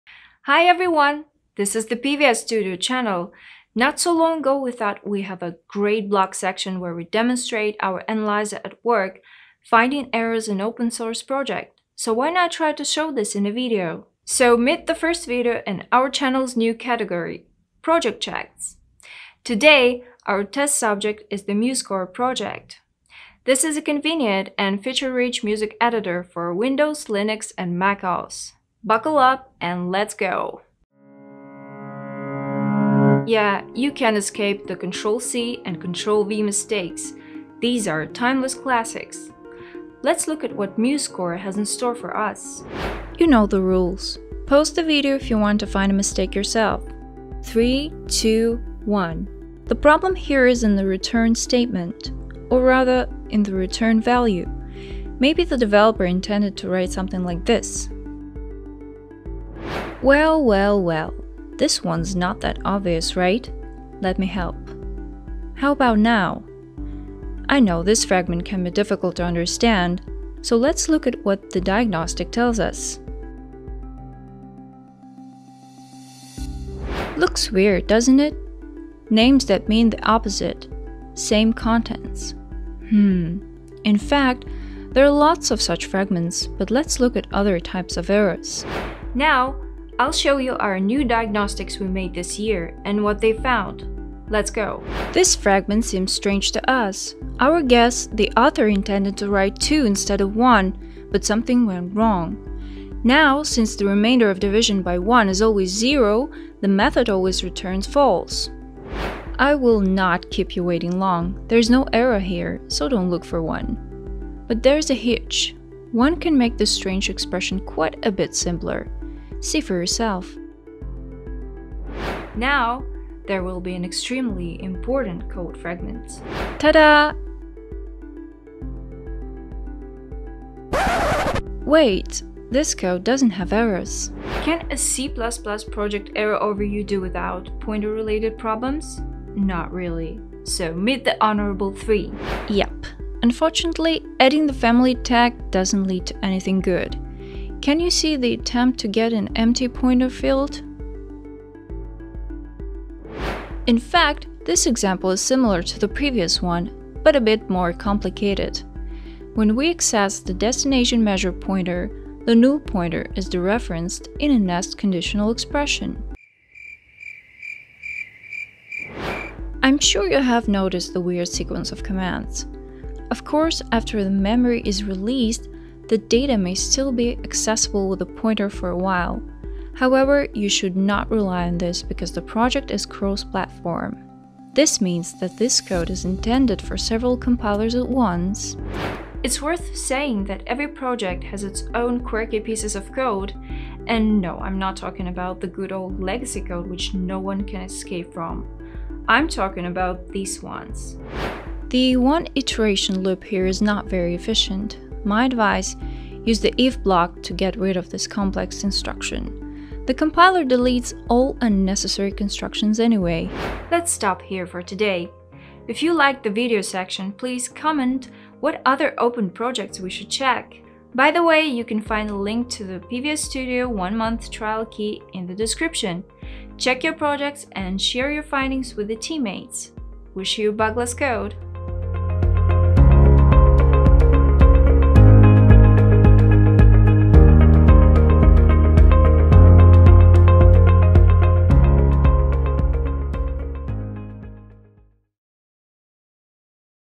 Short-lived music [MuseScore code analysis]
No project is perfect - whichever open-source project you examine, you can find a bug, or two - or... Welcome to our new video series where we study cases like this one. Let's start with MuseScore: we downloaded and inspected the project's source code. Now sit back and relax while we talk about the peculiar code fragments we found.

00:00 Intro
00:33 Copy-paste time
01:28 A couple of new diagnostics
02:01 An extremely important code fragment
02:14 Timeless classics: pointers in C++
03:09 Icing on the cake
03:33 Conclusion

Have fun watching this video and coding :)